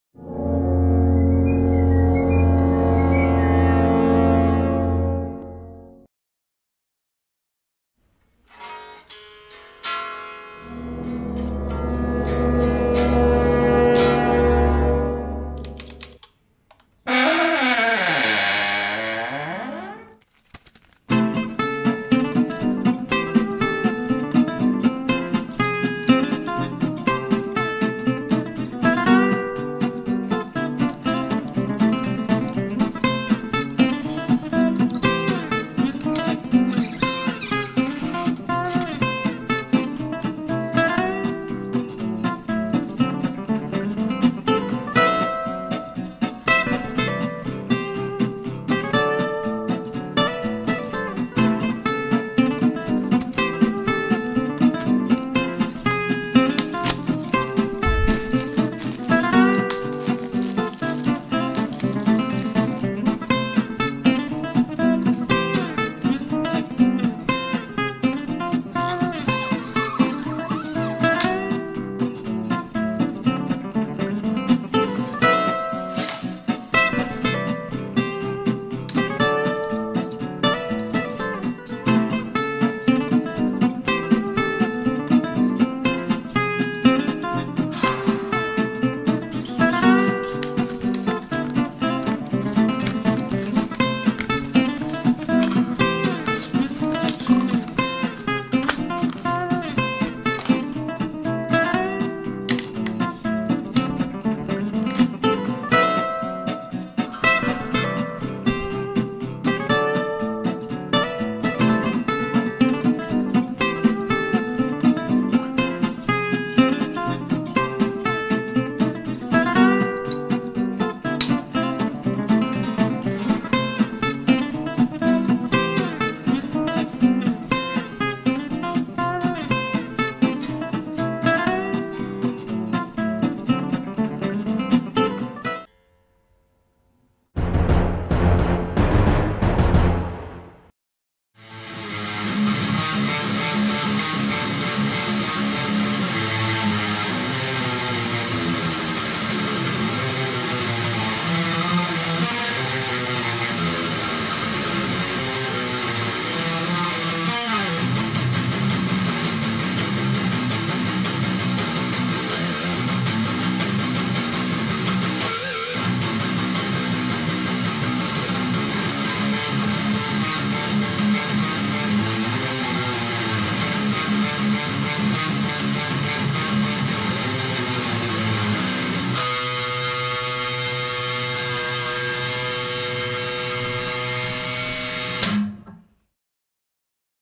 Palit-Kwerdas (String change)
Palit-Kwerdas (Guitar String change)

I was bored 1 saturday afternoon & decided to film my string replacement.
Shot using ZOOM Q3HD, edited through iMovie.  Enjoy!

gears used: Greco LP 70s model, Korg Pitch Black, D'Addario strings, Line 6 Spider 3 amp.